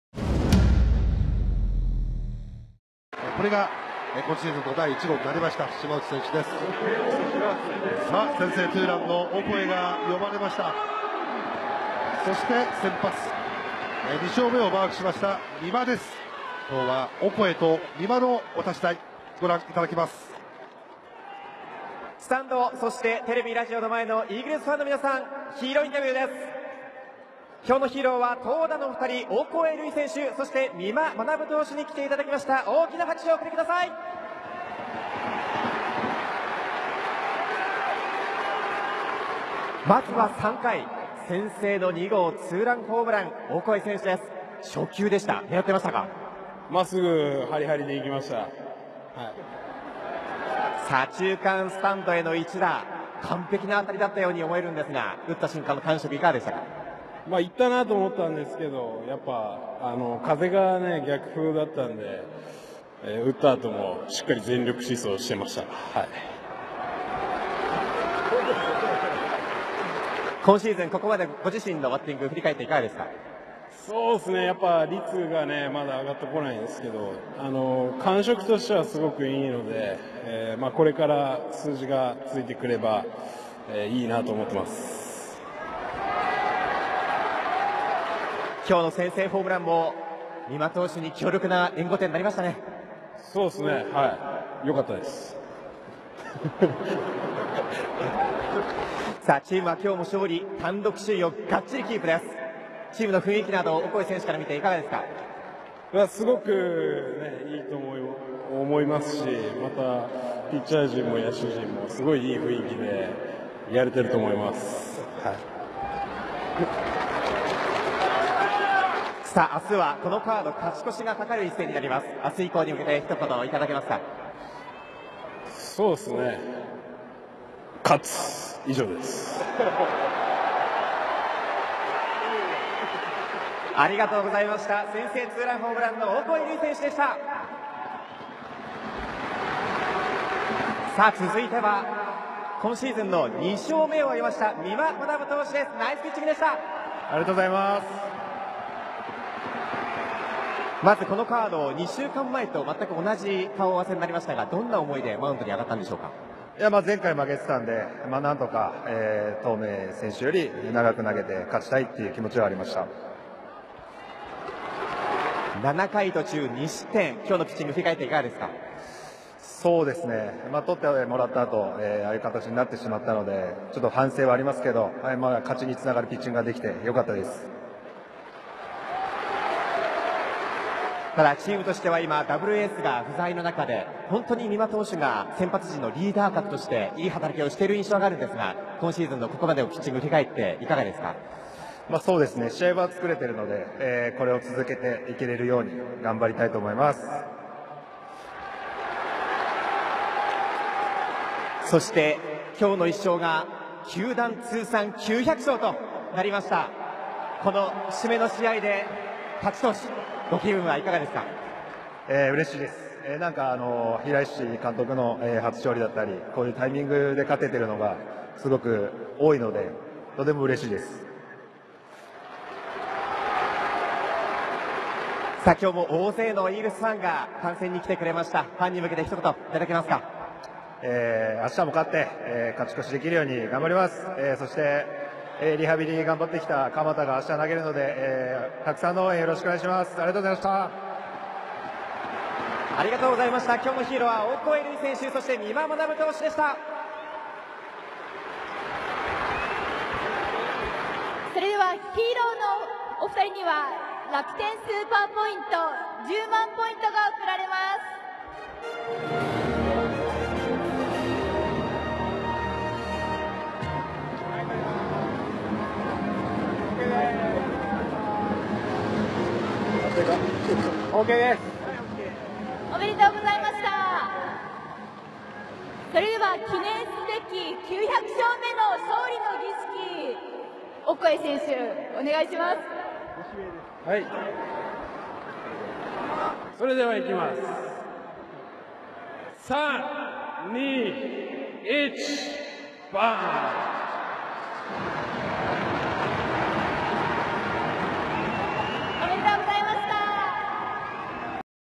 2019年4月20日 東北楽天・オコエ選手・美馬投手ヒーローインタビュー
イーグルス・オコエ選手・美馬投手のヒーローインタビュー動画。 2019/04/20 東北楽天ゴールデンイーグルス 対 オリックス・バファローズ